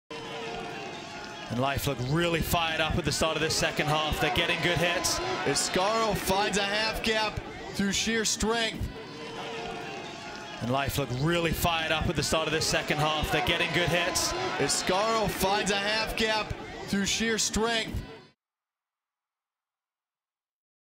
Crash Ball Half Gap: LIFE UNIVERSITY v UNIVERSITY OF CALIFORNIA (Jack Iscaro) D1A 2019 Final 5-14-19
CRASH BALL HALF GAP 1